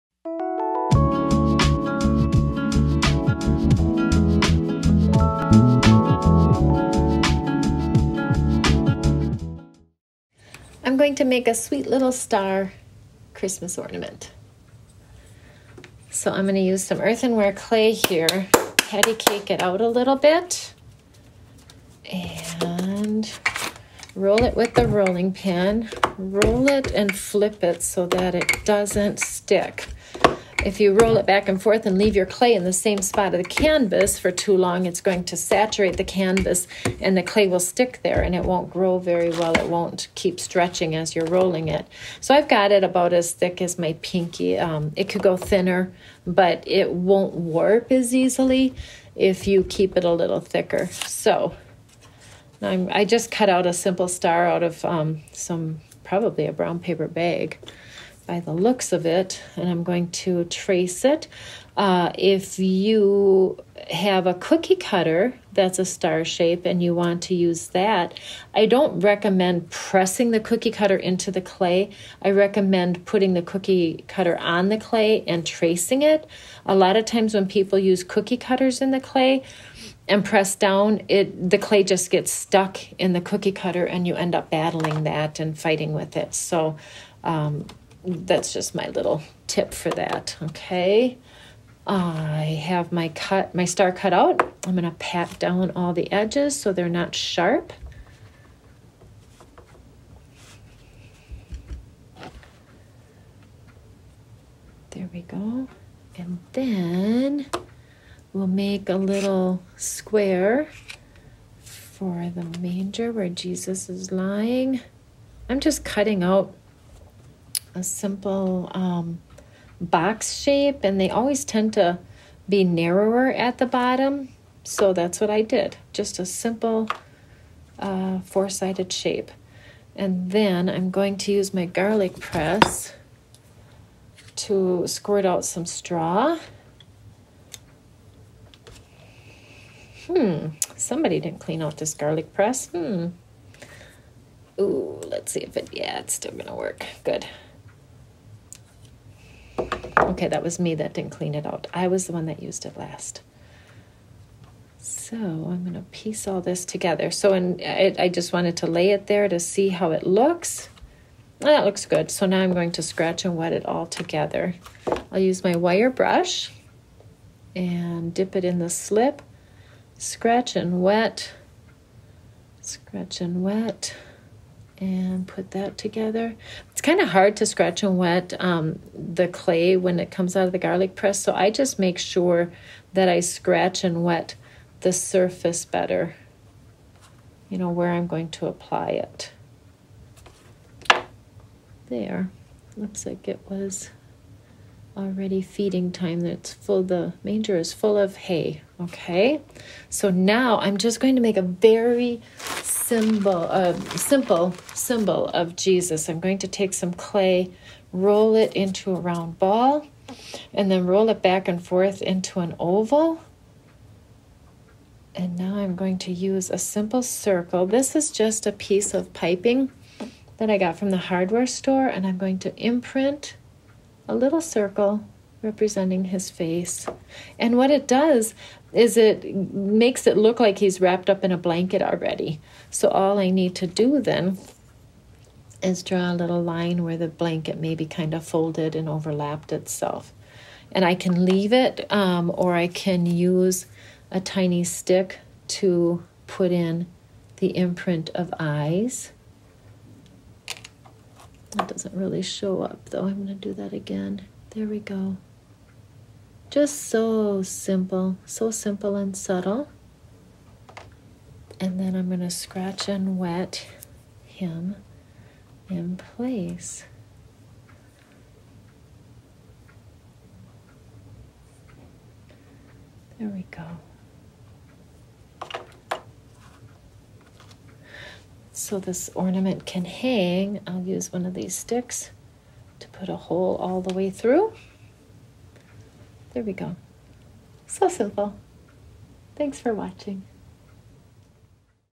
Clay Star Ornament Sculpting Video Lesson
This tutorial will show you how to make a Christmas Star with clay using hand building and sculpting techniques

Please check out my facebook/instagram and website for more captivating information about creating pottery

Instagram name
Lakecountryfinearts